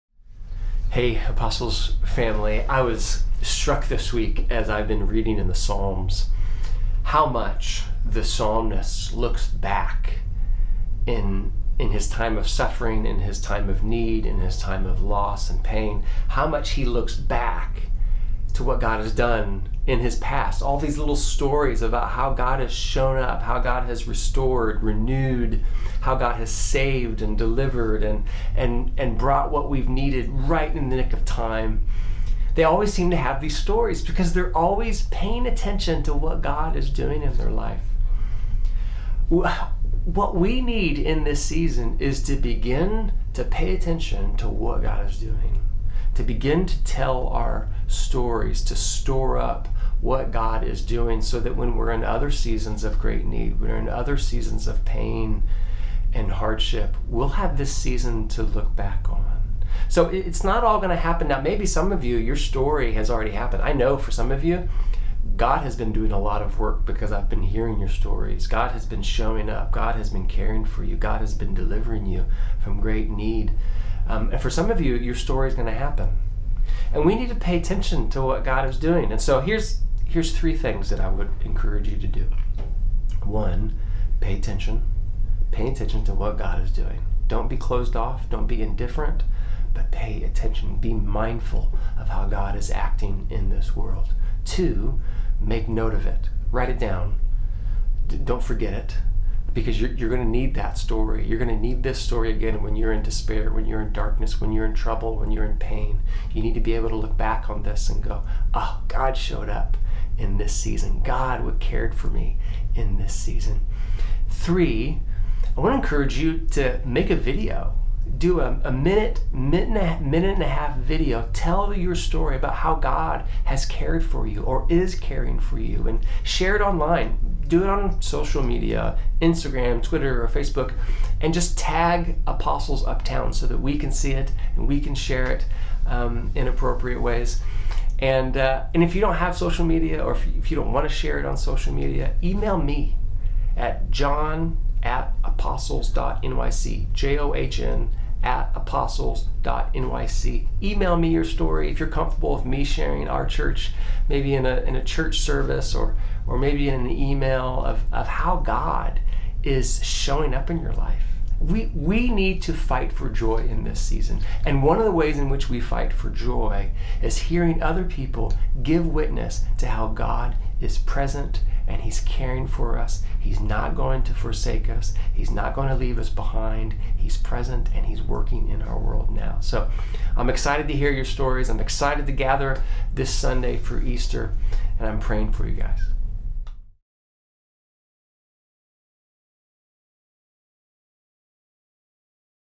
Share Your Story
John Starke gives a word of encouragement to share our stories of how the Lord has worked during this season.